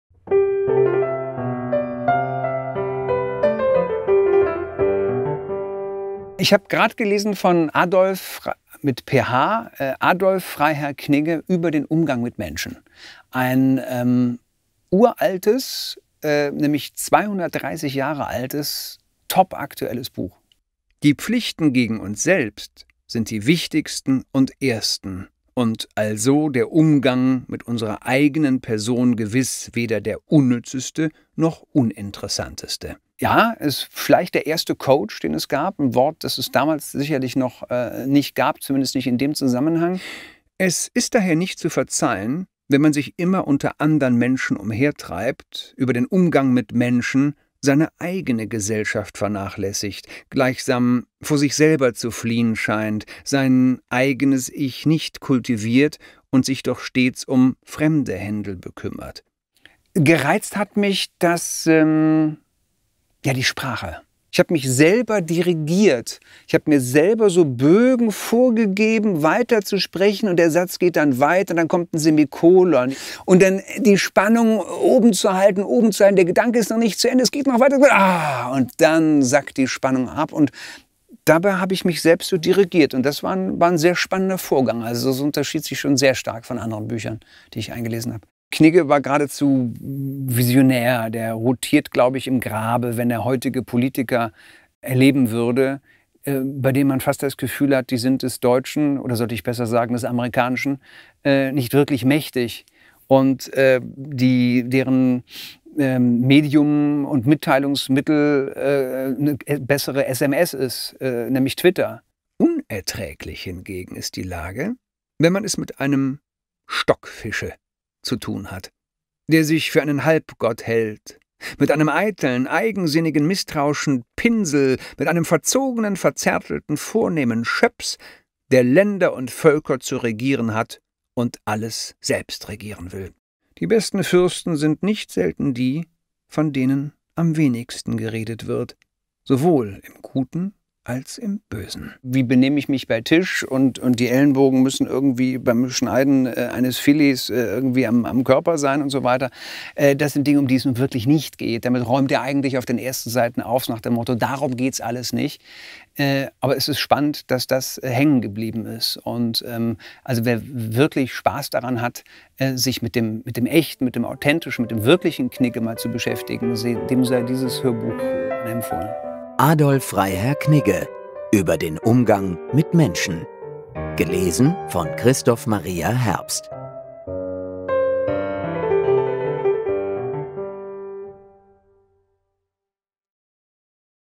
Christoph Maria Herbst erzählt über Knigge und sein Hörbuch "Knigge – Über den Umgang mit Menschen"
Das Hörbuch erscheint am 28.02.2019

»Was sagt denn der ›Knigge‹ dazu?« Ja, was eigentlich? Bis heute gilt der Knigge als wichtige Instanz bei Fragen des korrekten gesellschaftlichen Benehmens. Das Ziel des Freiherrn Knigge, den Menschen einen anständigen Umgang miteinander beizubringen, hat von seiner Aktualität nichts verloren. Wenn man also wissen will, wie man sich Freunden gegenüber verhält, mit Herrschsüchtigen umgeht, oder über welche Themen man in Gesellschaft besser nicht redet, dann ist man beim Knigge nach wie vor an der richtigen Adresse. Und wer könnte diese Sammlung der besten Knigge-Weisheiten besser interpretieren als Christoph Maria Herbst? Richtig: niemand.

Video produziert von Spotting Image, Köln.